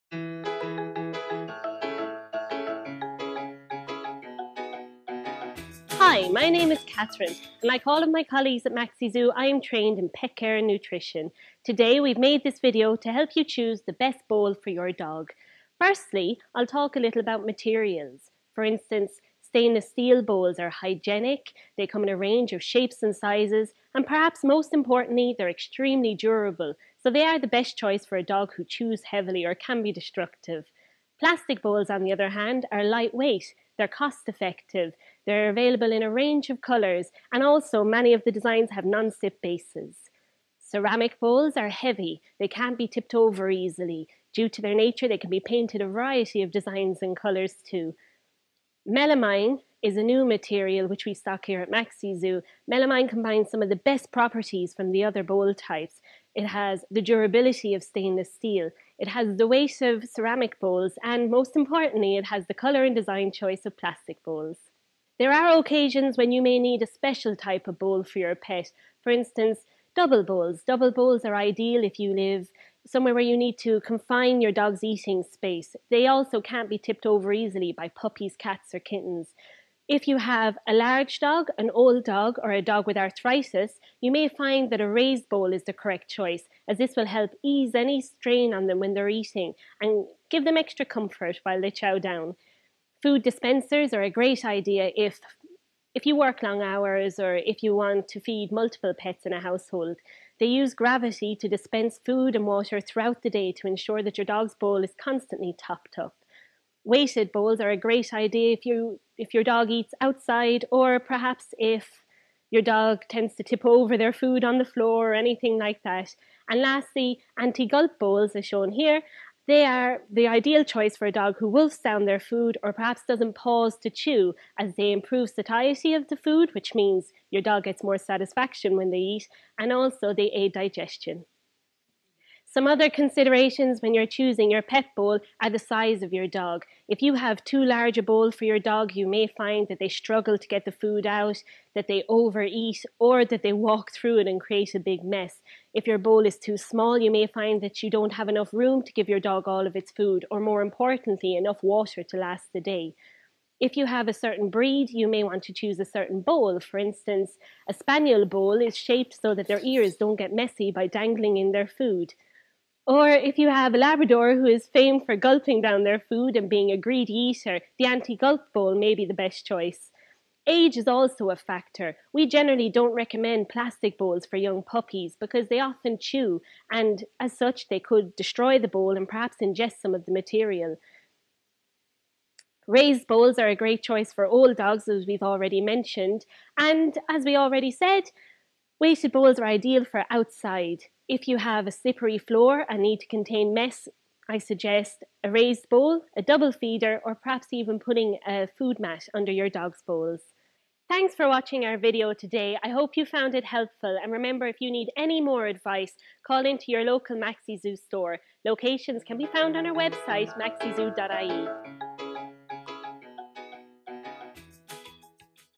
Choosing A Dog Bowl - Maxi Zoo Ireland
With such a wide range of dog bowls available which one is the right choice for your dog? .Check out this video for lots tips and information on what is available in your local Maxi Zoo store.